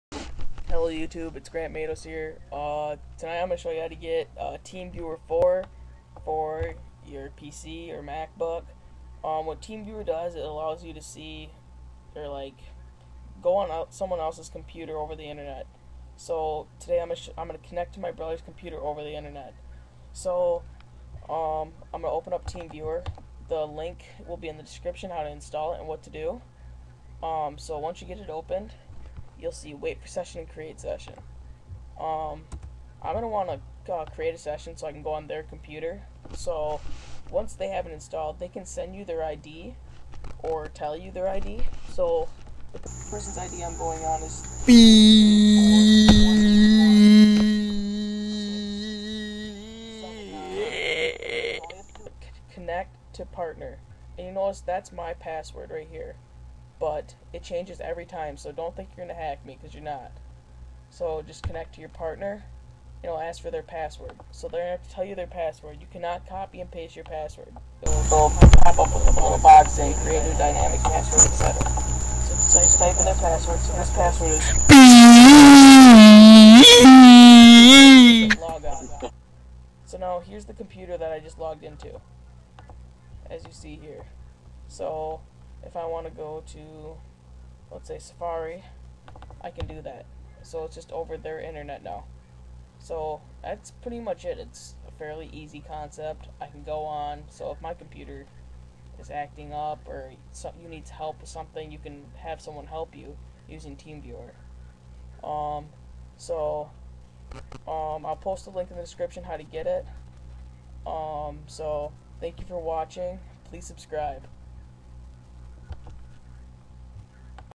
How To Remotely Access Your Computer
This is a tutorial on how to remotely access your computer using TeamViewer.

Use it for personal use only!